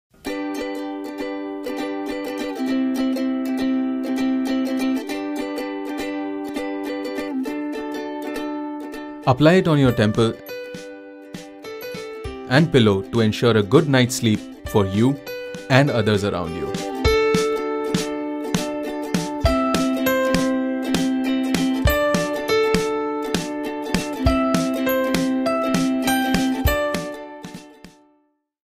how to use snore solution |soulflower
ABOUT ANTI SNORING AROMATHERAPY ROLL ON
Try this blend to help get snoring problems under control. This blend is designed to substantially reduce snoring by reducing the tension and stiffness of the throat muscles, thinning out mucous and by lubricating the tissues in the mouth and throat. Snoring is caused by vibration of the soft tissue in the back of the throat. Anti -Snoring blend helps to open the nasal passage. Its astringent properties help to reduce the movement of soft tissue, through contracting effect, helping to eliminate snoring.
Signature Roll-on. Great for travel
Infused with anti-oxidants this nutrient-enriched oil blend nourishes skin with Vitamins A, B, C & E
while stimulating the senses instantly
Base of cold pressed jojoba and olive oil & mineral Vitamins